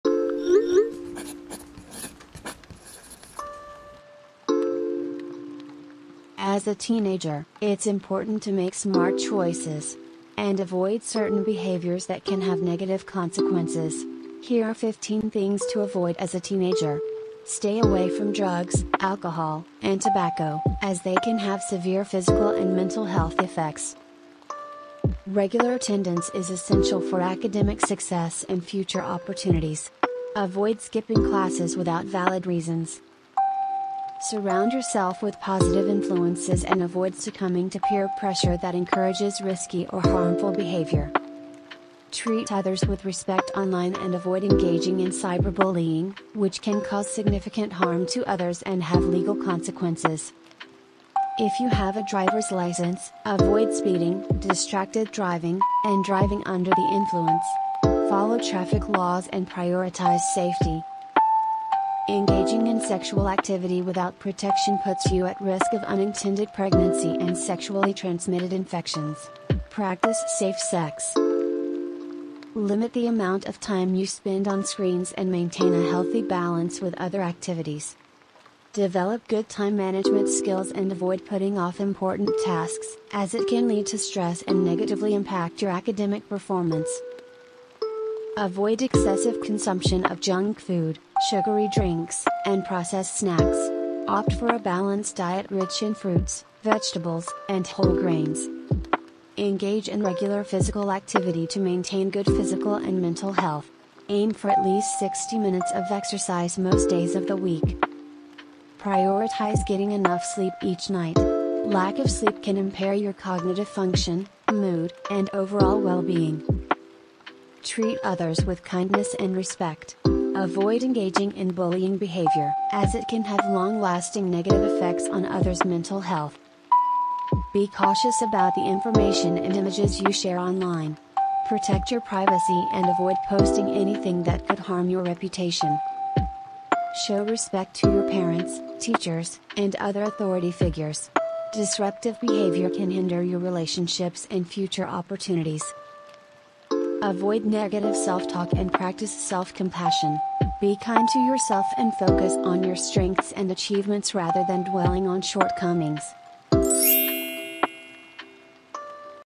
💗Things to AVOID as a teenager ||. Bad Habits || Habits to stop 🛑🚫
🌷Being a teenager is a time of growth and self-discovery. ✨By avoiding these negative behaviors, you can set yourself up for a healthier and more fulfilling future.❇️

❣️I Hope you liked the video and this was helpful to you.